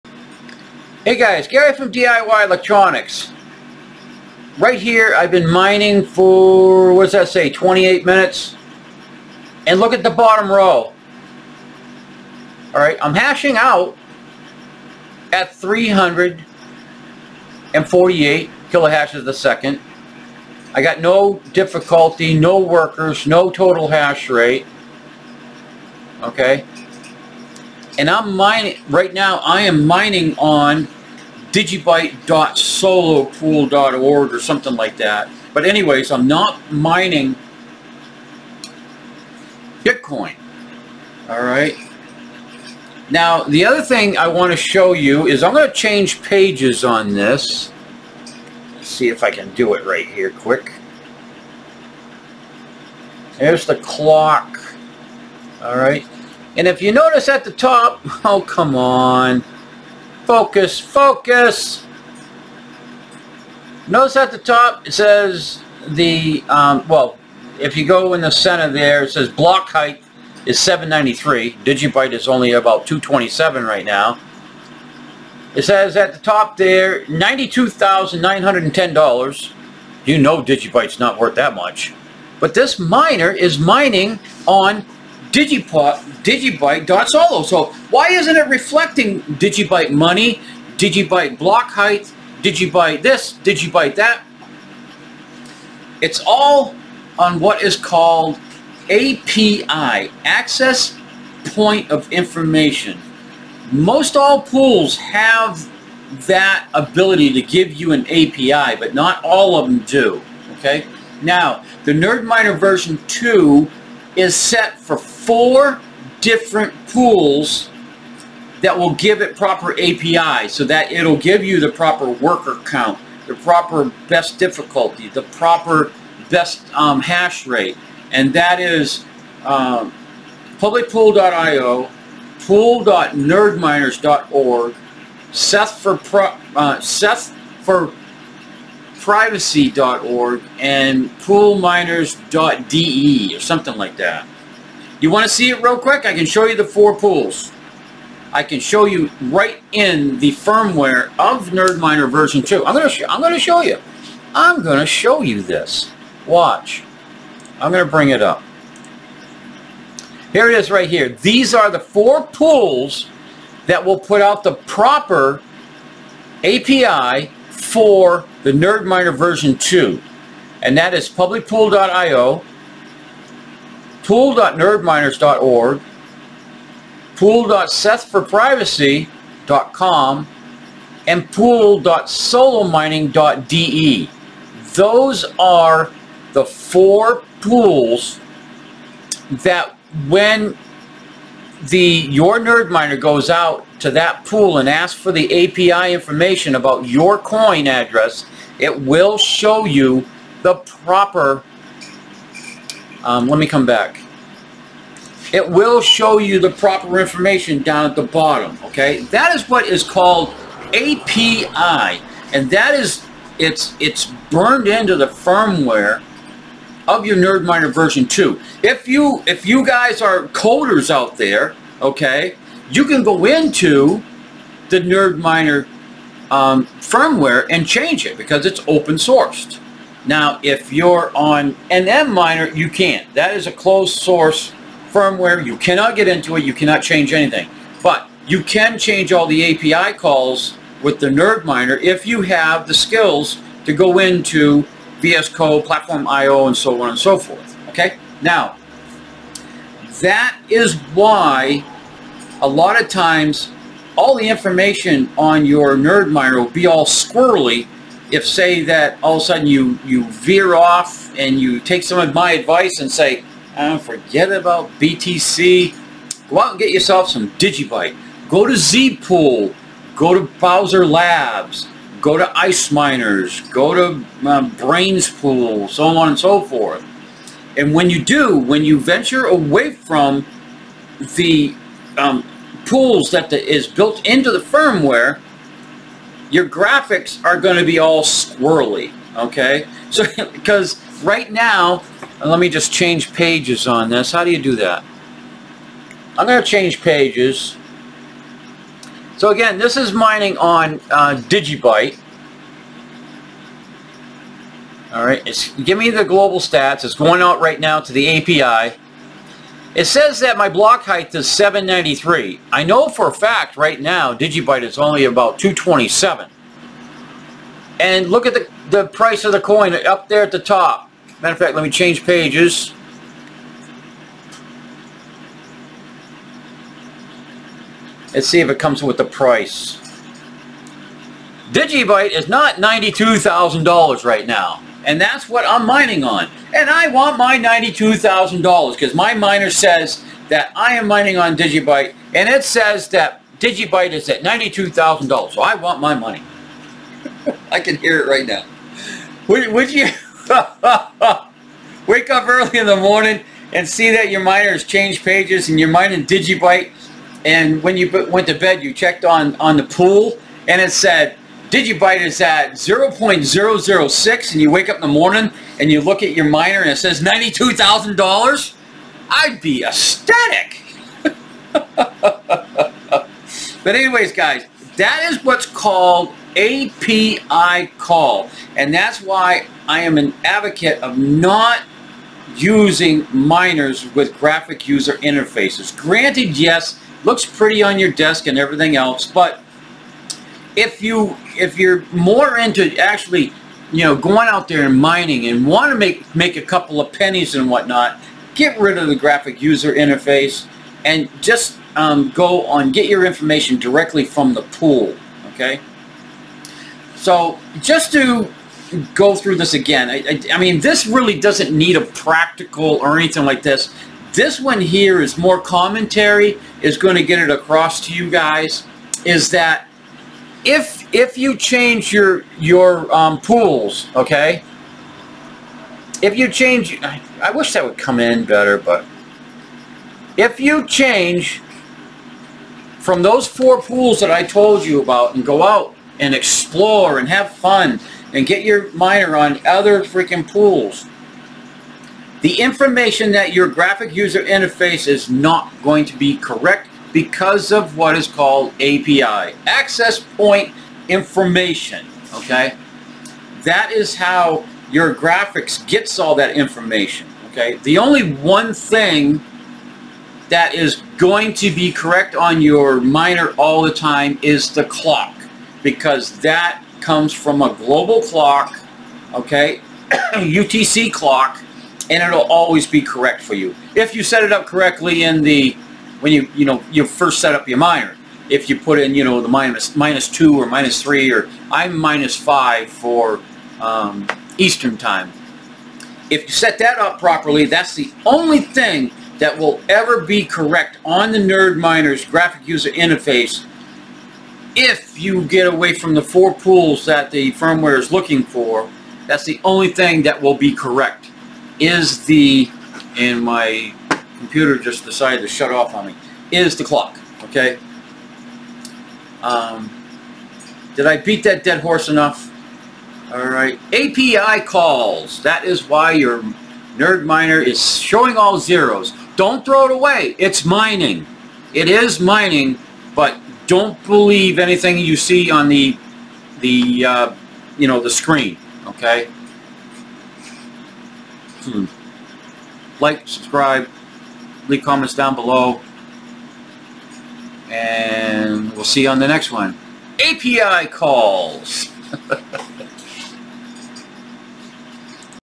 Why NerdMinerV2 Stats Look Wrong on Custom Pools, API Explained
In this video, I explain how the NerdMinerV2 gets the information you see on the web GUI.
And why switching to pools that the firmware was not designed for can cause missing or
Incorrect stats.

Oooo. Coffee sounds good right about now, but DGB is down. Can you spare a cup for this old YouTuber.

The NerdMinerV2 does NOT calculate most of the numbers you see on the screen.
Those values come from the mining pool’s API.

If a pool uses a different API format, different variable names, or no API at all,
The GUI may:
- Show incorrect hashrate
- Freeze numbers
- Show zero shares
- Display missing or broken stats

This does NOT mean the miner has stopped hashing.

The firmware is written to expect specific pool APIs.
When you stray away from those pools, the miner will still work,
but the GUI can no longer interpret the data correctly.

This video is meant to clear up confusion and stop the myth that
"GUI problems = lost money.

If you are new to NerdMinerV2 or ESP32 mining,
This is important information to understand before changing pools.

Cheers 🍻